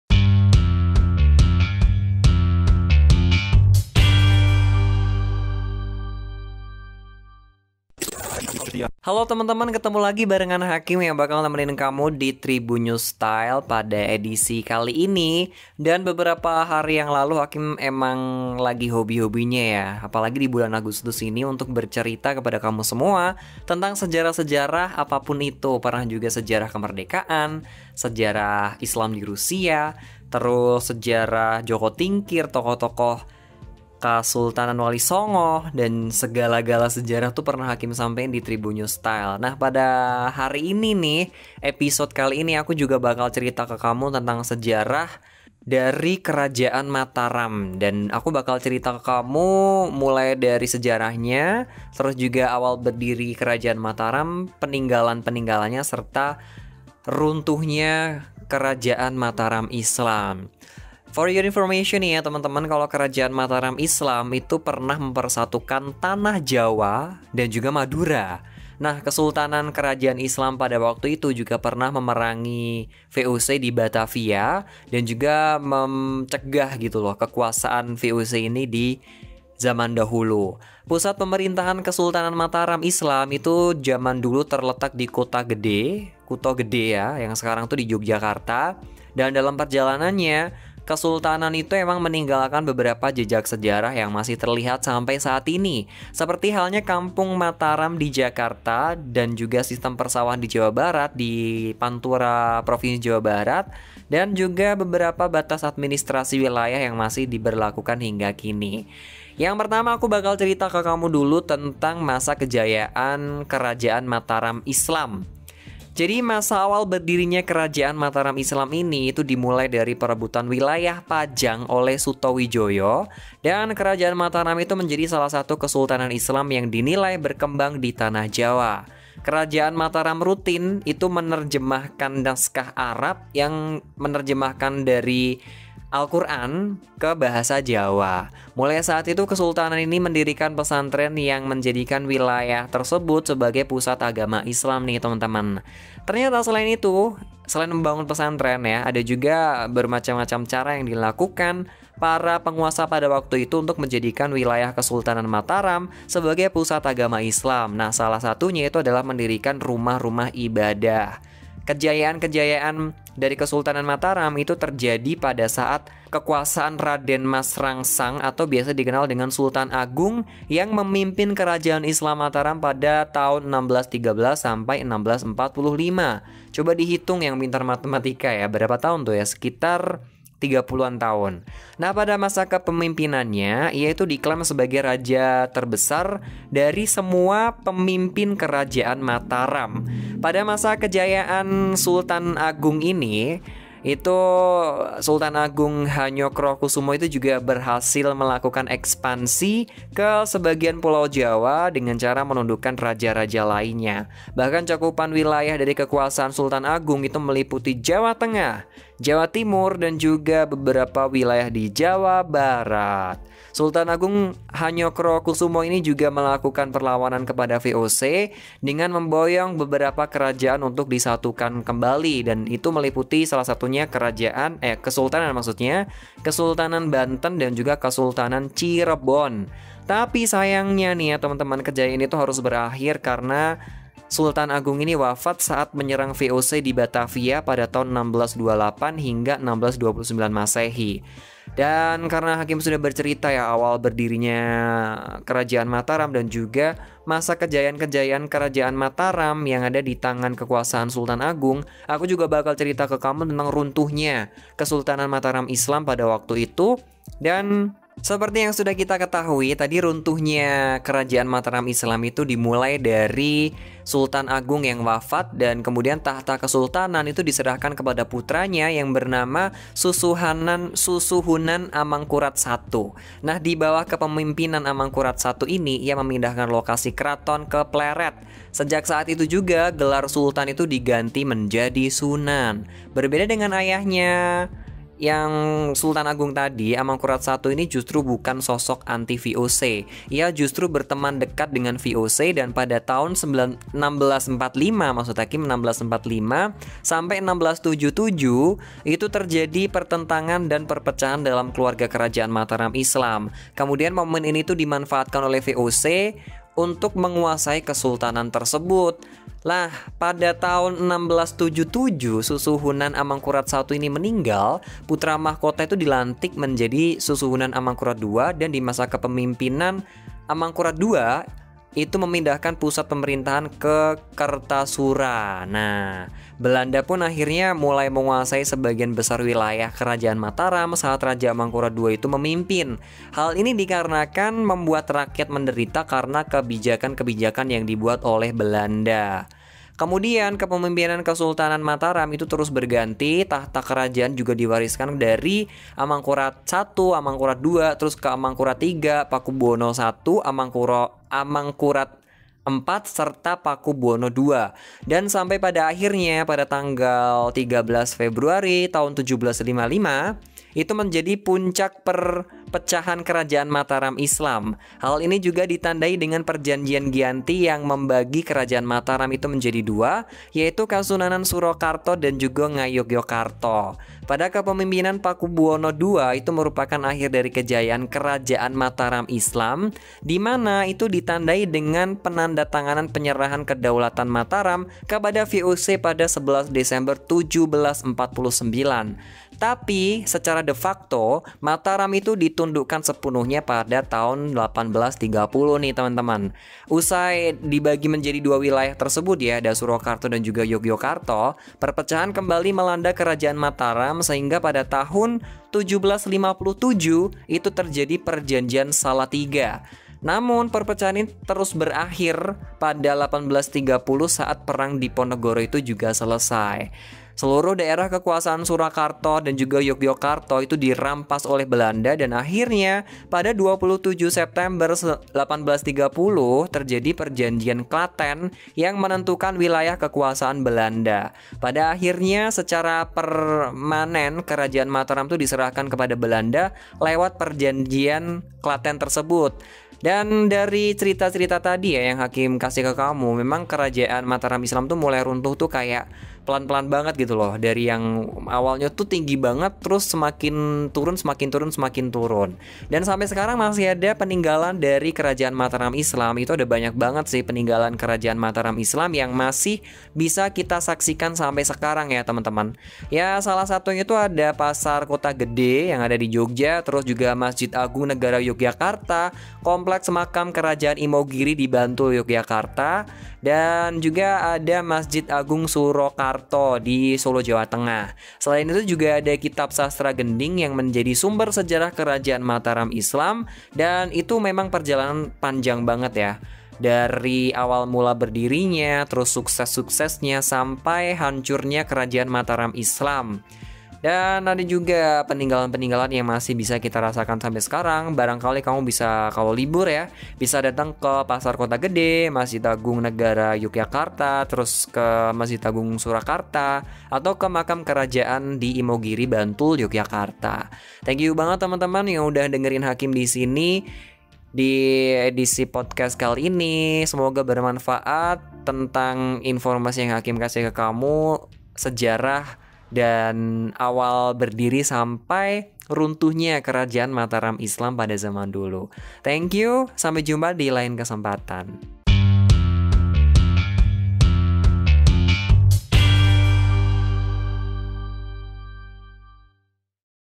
Kerajaan Mataram: Sejarah, Kejayaan hingga Runtuhnya Mataram Islam
TRIBUN-VIDEO.COM - Kesultanan ini merupakan kerajaan berbasis pertanian, dengan menerapkan ajaran Islam dalam perjalanan kejayaannya. Berikut perjalanan sejarah Kerajaan Mataram Islam, dari masa awal berdiri hingga runtuhnya kesultanan tersebut.

Simak selengkapnya Tribunnews Podcast di